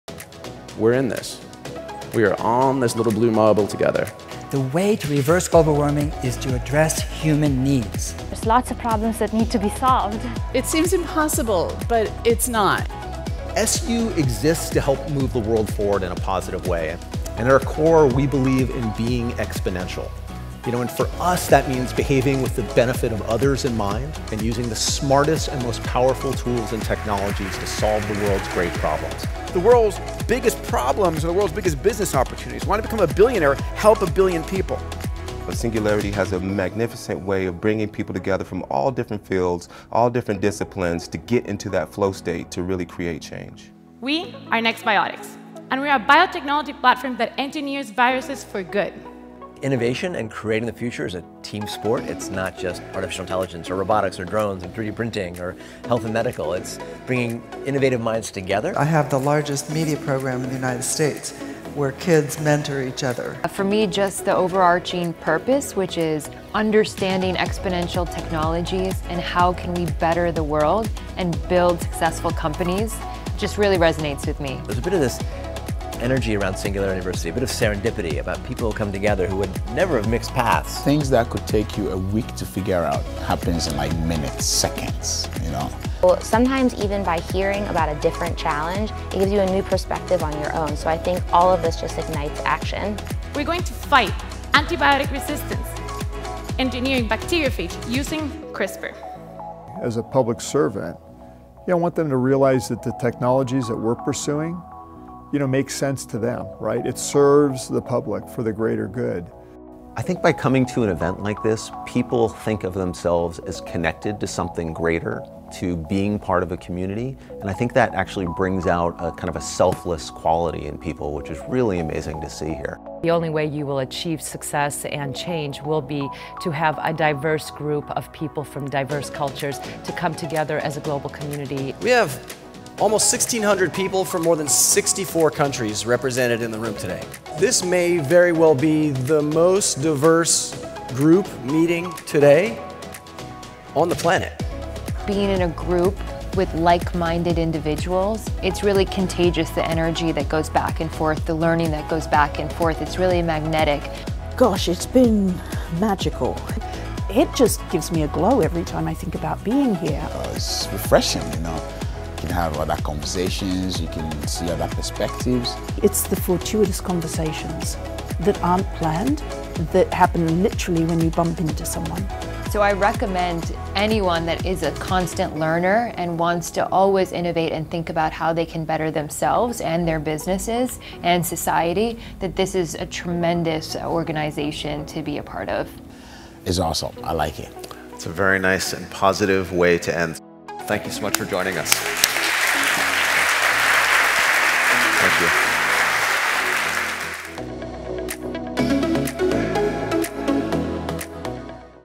Singularity University Global Summit 2018
Join us in San Francisco August 20-22, 2018.

SU Global Summit brings together a world-class collection of leaders. Together, we strive to better understand the future through exponential technologies, create meaningful connections among colleagues, and empower every individual to go out and build a better world. This event is the definitive annual gathering of the Singularity University community, and the most impactful moment of connection for a global network of innovators.